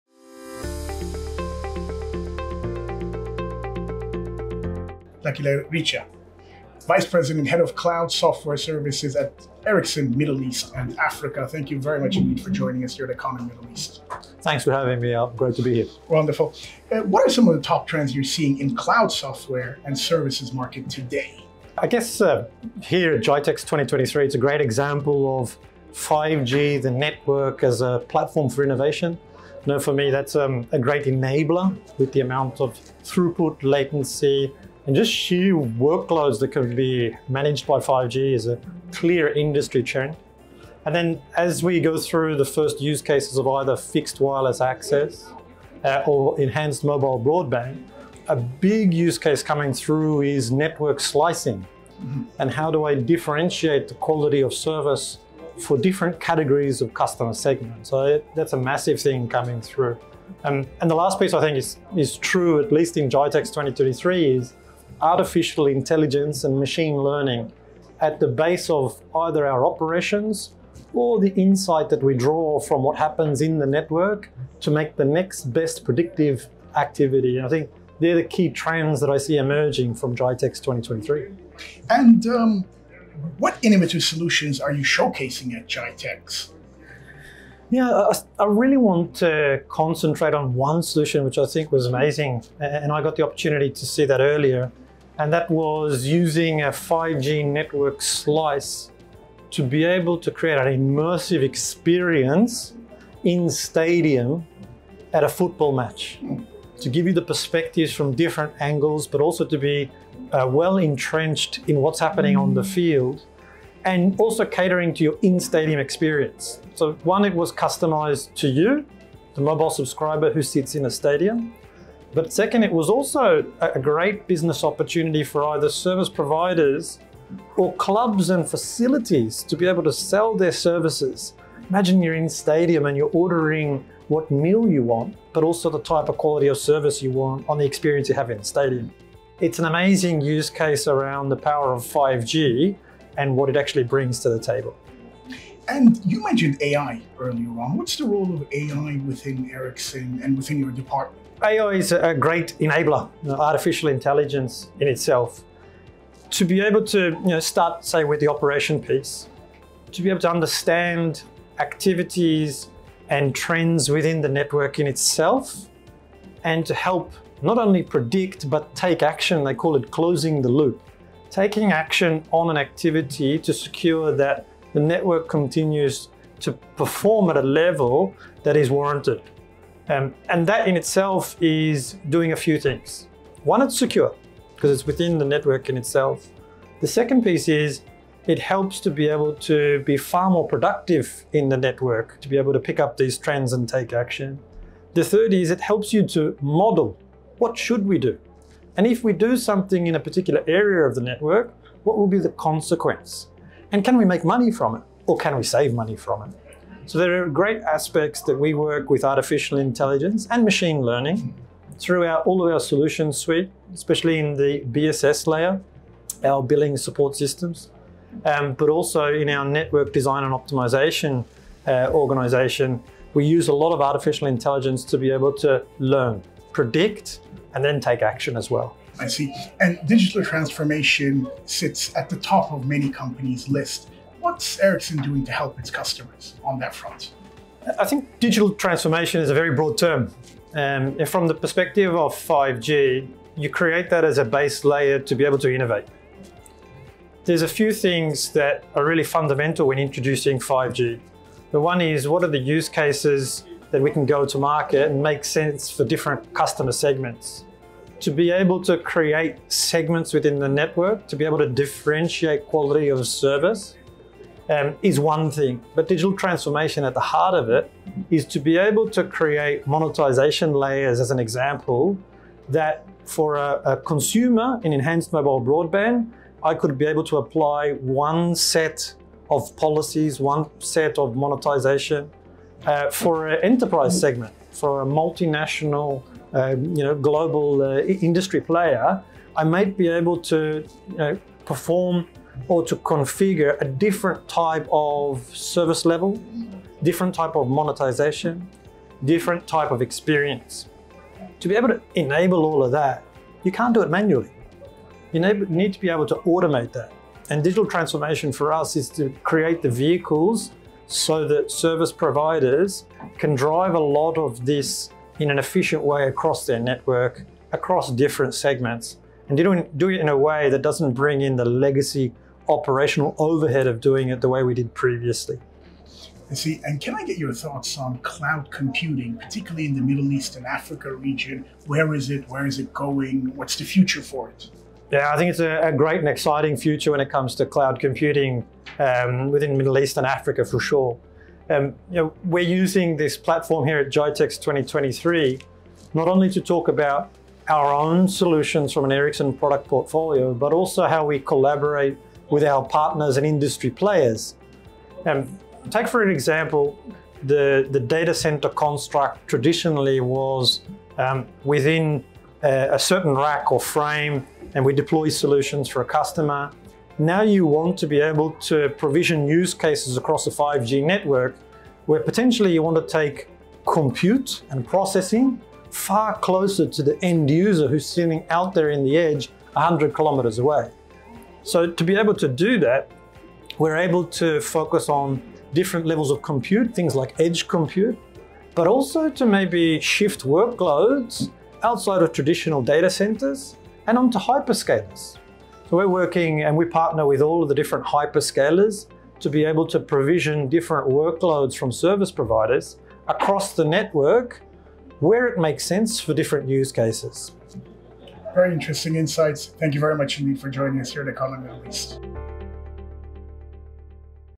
Interview with Lucky La Riccia, VP & head of cloud, software & services at Ericsson MEA
At GITEX GLOBAL 2023, Lucky La Riccia, vice president and head of cloud, software and services at Ericsson Middle East and Africa offers a compelling presentation highlighting the cutting-edge solutions that Ericsson is unveiling at the event. With remarkable insight, he explores the current dominant trends in the ever-evolving cloud, software, and services market. In addition, La Riccia elaborates on Ericsson's strategic vision to expedite the secure digital transformation endeavors of their clientele. He concludes by sharing his perspective on the future of cloud computing within the Middle East and Africa region.

Chapters:
Trends in the cloud, software, and services market: 00:00 – 01:21
Ericsson Innovative solutions: 01:22 – 02:25
The role of AI within Ericsson: 02:26 – 03:49
Customers digital transformation journey 03:50 - 05:26
Cloud Computing in Middle East & Africa 05:27 – 06:51